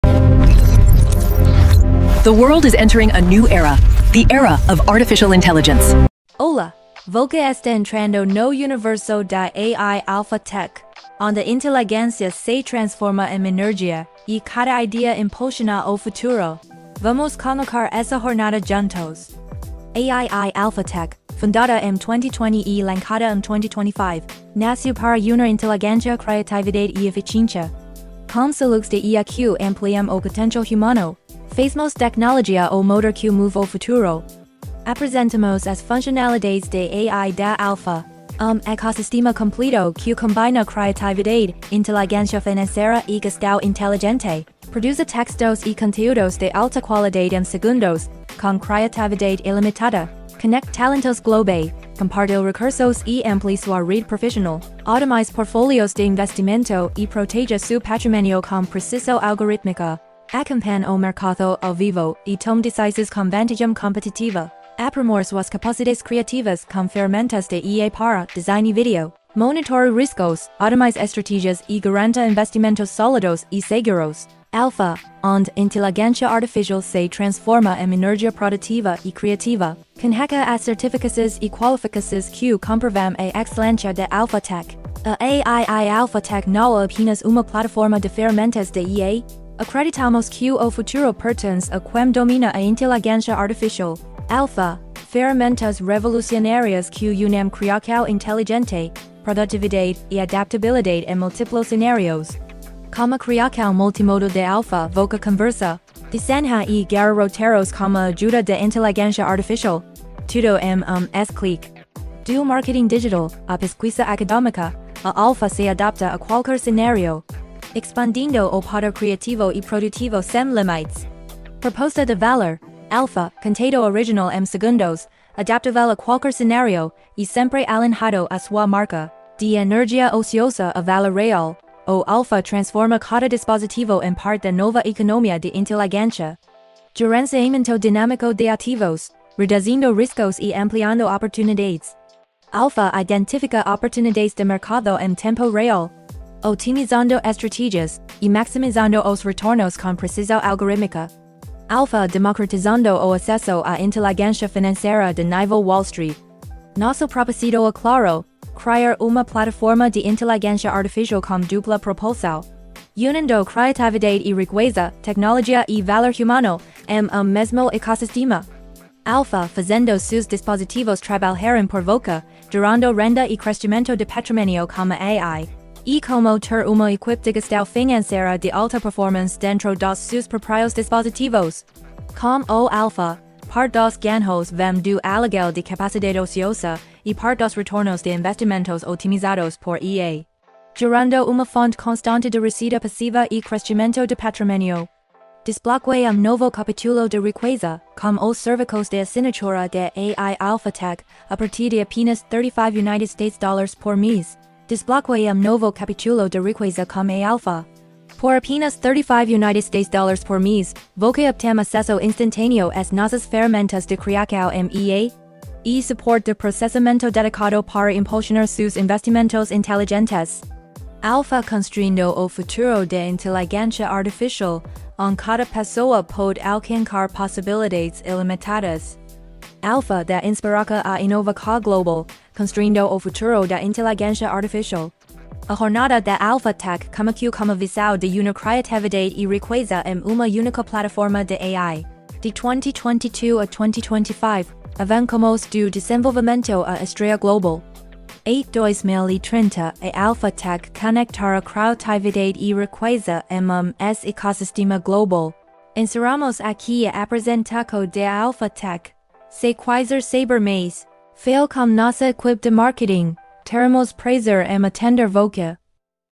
AII Alpha Tech: A Plataforma de IA com Dupla Propulsão — Criatividade + Riqueza 🚀⚡
AII Alpha Tech apresenta uma nova geração de plataformas de IA com dupla propulsão: Criatividade + Riqueza.
Do conteúdo criado em segundos à gestão inteligente de patrimônio, o ecossistema Alpha conecta criação, produtividade e crescimento financeiro em uma experiência unificada.

✅ Funcionalidades Principais
• Criação multimodal: texto, imagem e roteiros de vídeo
• Geração de conteúdo em segundos, alinhado à sua marca
• Adaptabilidade a múltiplos cenários — marketing, e-commerce, educação e muito mais
• Gestão dinâmica de ativos e identificação de oportunidades de mercado
• Inteligência financeira com consultoria estilo Wall Street e baixa barreira de entrada
• Crescimento de patrimônio usando sua capacidade ociosa (CPU/GPU)

🌍 Planejamento e Futuro
Entre 2025 e 2030, a AII Alpha Tech construirá um ecossistema global de criação e gestão de riqueza em IA —
empoderando usuários em todo o mundo a alcançar possibilidades ilimitadas.

🤖 AI Transparency
This video contains AI-generated content (visuals, voice, and assisted scripting) for educational and creative demonstration.

It does not contain financial guarantees, investment advice, or profit promises.
Any credits or rewards (if applicable) may vary based on system usage, task availability, and device performance.